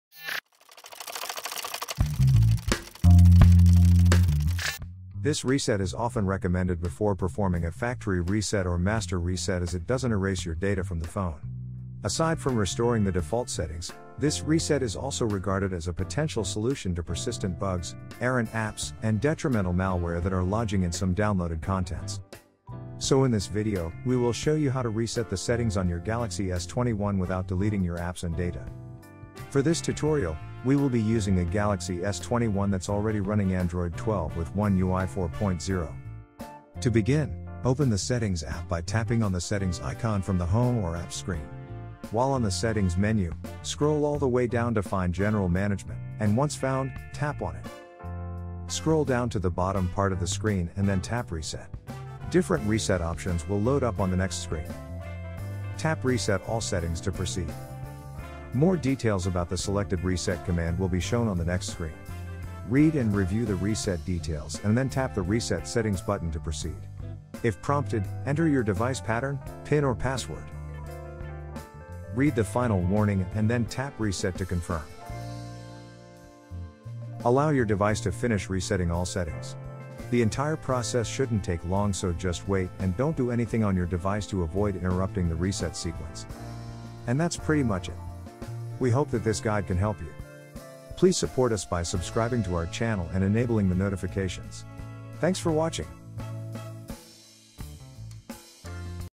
How to Reset All Settings on Samsung Galaxy S21 without Deleting User Data (Android 12, One UI 4.0)
This reset is often recommended before performing a factory reset or master reset as it doesn’t erase your data from the phone.

Aside from restoring the default settings, this reset is also regarded as a potential solution to persistent bugs, errant apps, and detrimental malware that are lodging in some downloaded contents.

So in this video, we will show you how to Reset the settings on your Galaxy S21 without deleting your apps and data.

For this tutorial, we will be using a Galaxy S21 that’s already running Android 12 with One UI 4.0.